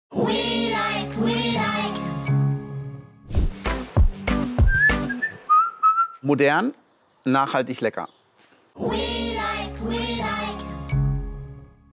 Frage an Max... Wie würdest du Velike! in drei Worten beschreiben?
Max Mutzke ist unser persönlicher Schwarzwald Held! 🌾🌲
Die perfekte Kombination zweier Originale aus dem Schwarzwald.

"Modern, nachhaltig, lecker!"

Folge Max Mutzke... 🎶

Folge Velike! ... 🌾

... um mehr über unsere Kooperation zu erfahren! Schau doch mal rein! 🌾🌲